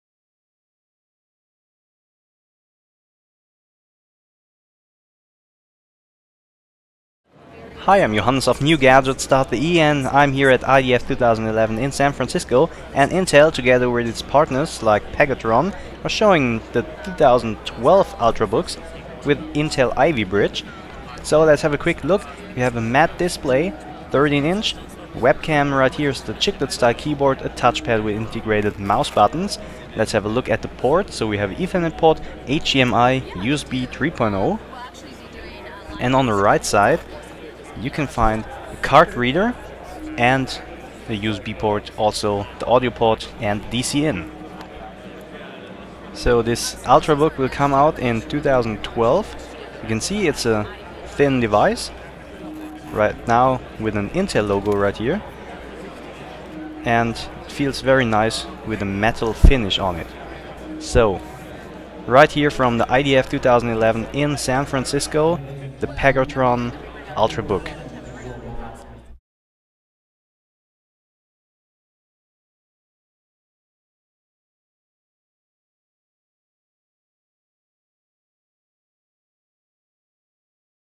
Hands On of the Pegatron 2012 Intel Ivy Bridge Ultrabook at IDF 2011
Hands On of the Pegatron 2012 Intel Ivy Bridge Ultrabook at IDF 2011. It uses a 1,5Ghz Intel Ivy Bridge Processor.